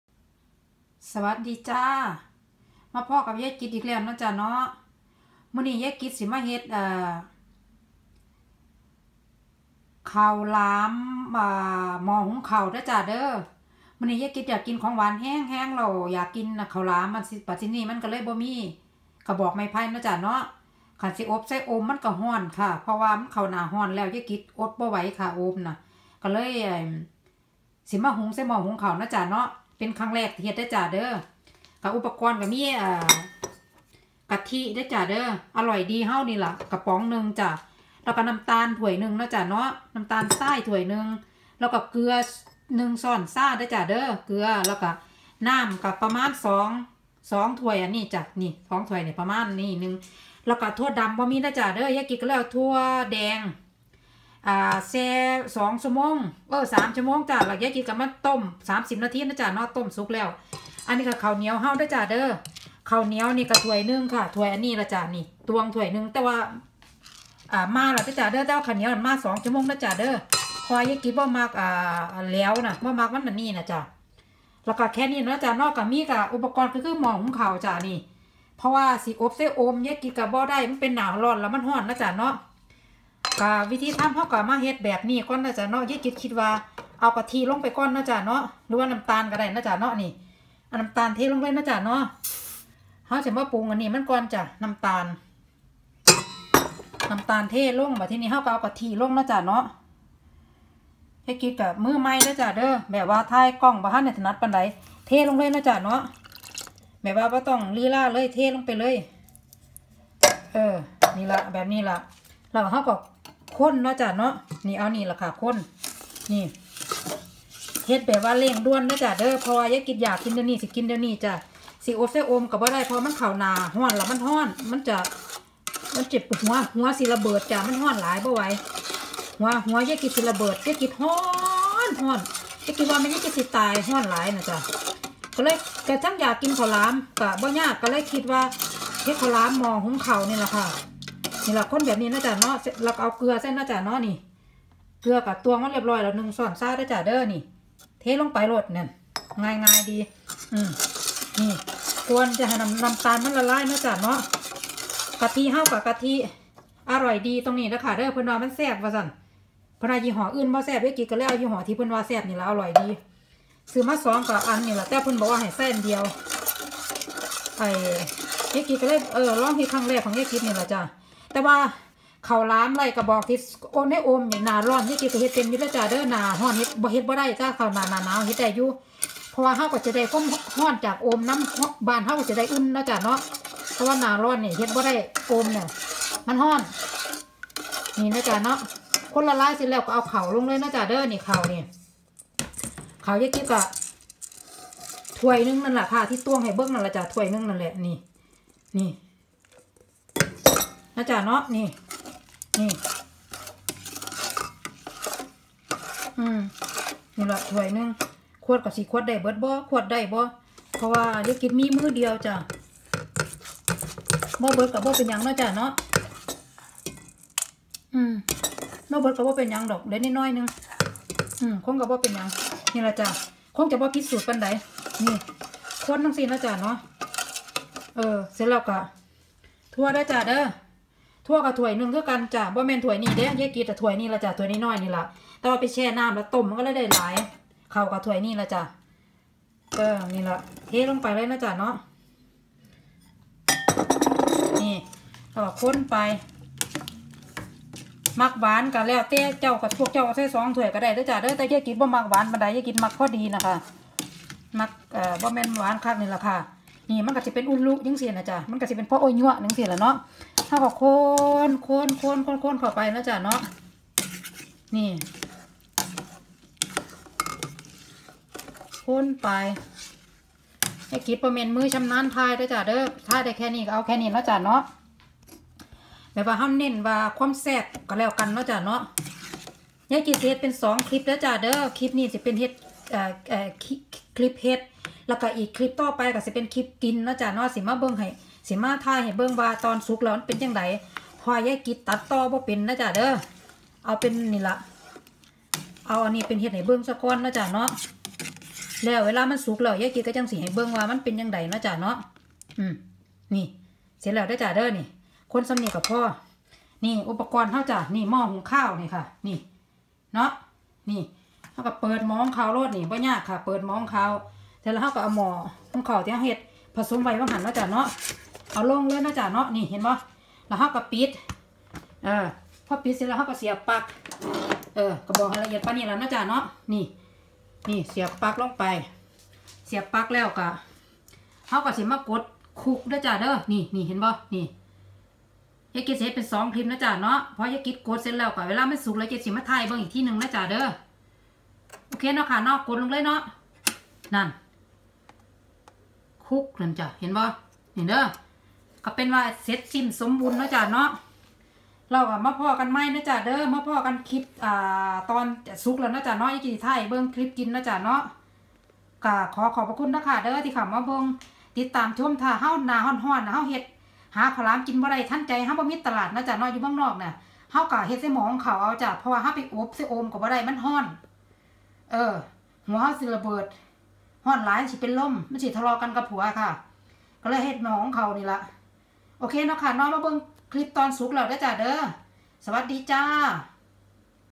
วิธีทำข้าวหลาม หม้อหุงข้าว ทำง่าย อร่อย
สวัสดีจ้าวันนี้ยายจิตมาทำข้าวหลามในหม้อหุงข้าว ทำง่าย อร่อย ยายจิต กินโชว์YouTube จ้า